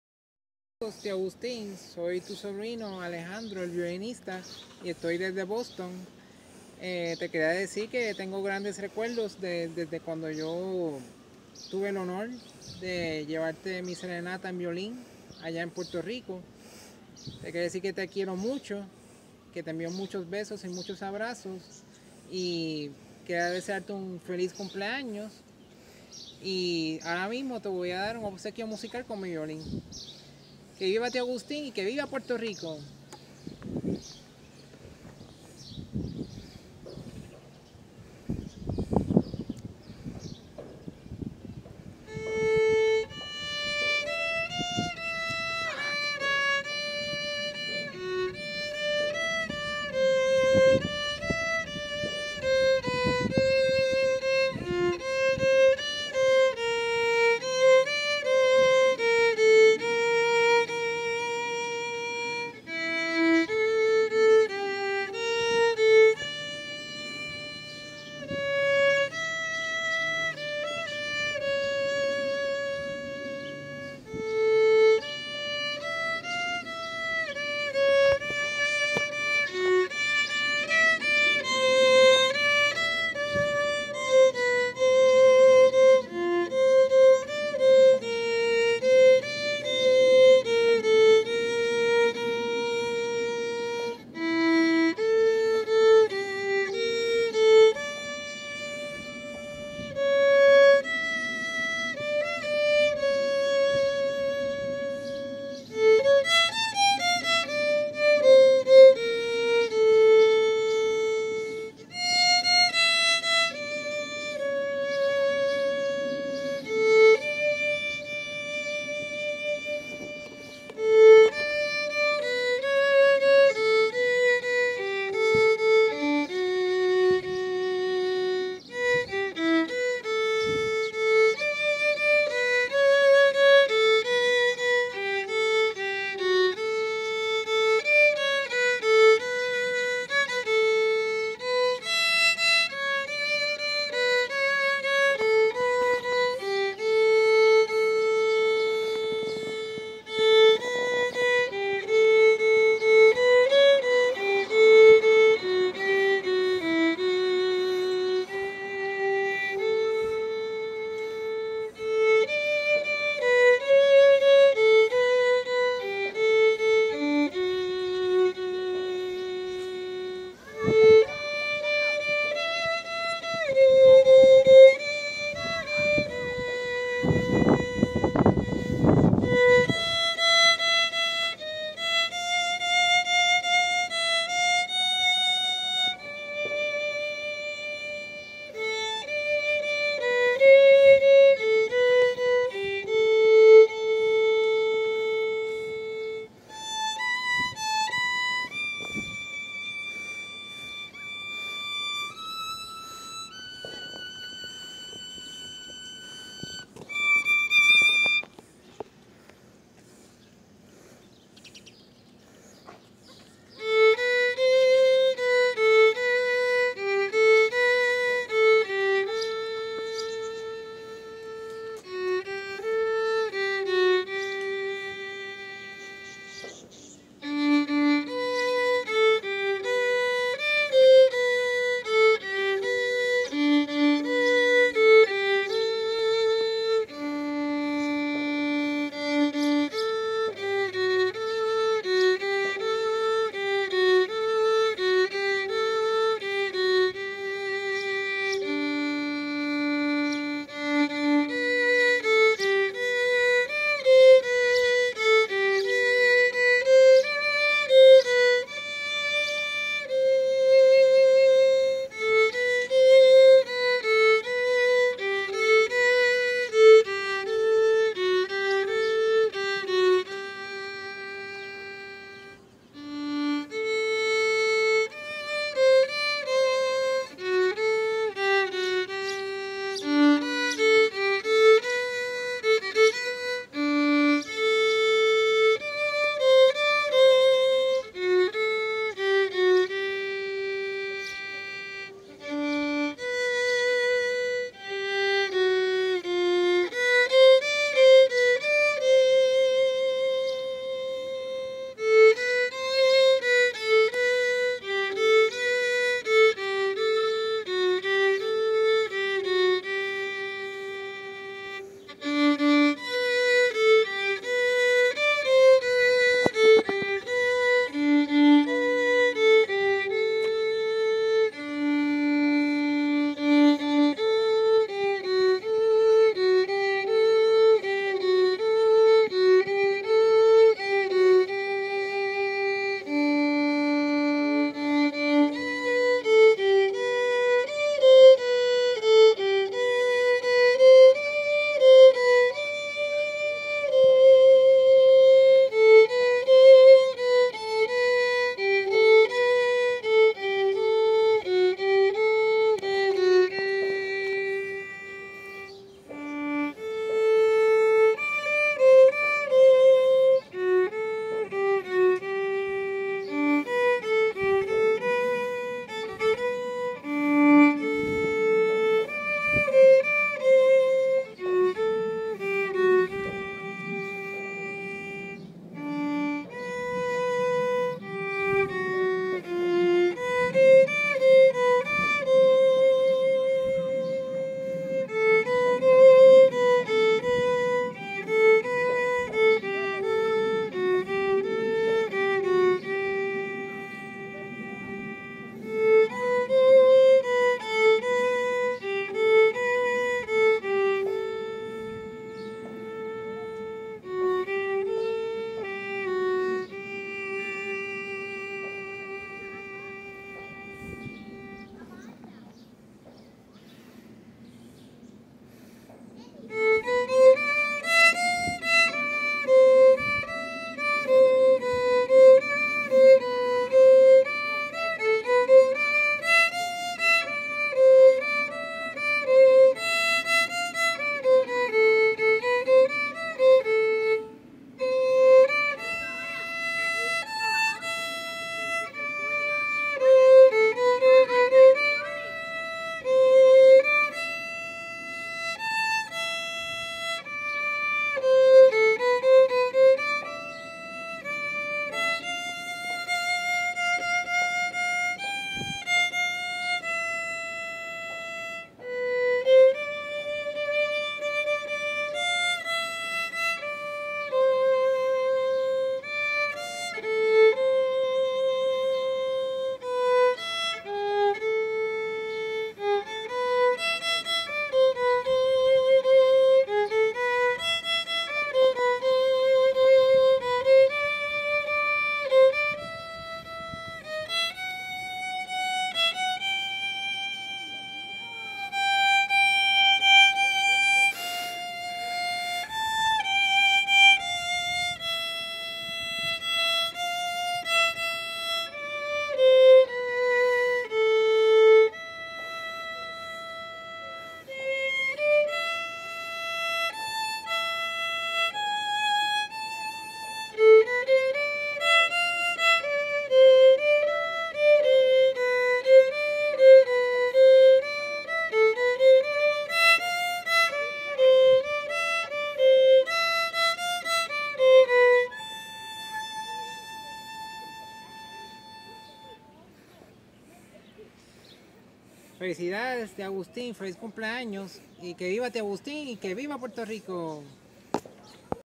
El Cumple 100 años de Tìo Agustín
Queridos Amigos hoy estamos celebrando en grande el cumpleaños número 100 de mi querido Tìo Agustín, ciudadano distinguido, matemático, educador de generaciones. Lo conocen como "La Gran Enciclopedia" por esa mente tan privilegiada que tiene. Cuando conversas con él, te sientes como si estuvieras en Un gran documental de National Geographic, de la NASA y del History Channel a la vez! Y con su luz brilla hasta El Infinito por su gran calidad Humana!! Luchador, valiente, digno, bondadoso. Posee grandes cualidades y me quedo corto con Todo lo que quiero expresar sober él. Hoy Tengo El honor y me llena de mucho orgullo El poder dedicarle mi humilde obsequio con estas piezas musicales en mi violin. Les pido de corazón que me acompañen en esta gran celebración El Cumple 100 de mi Tìo Agustín!! Celebremos todos juntos, matarile-rile-ron!!! Te quiero mucho Tìo! Que Viva Tío Agustín!! y que Viva Puerto Rico!! ❤️🇵🇷🙏 //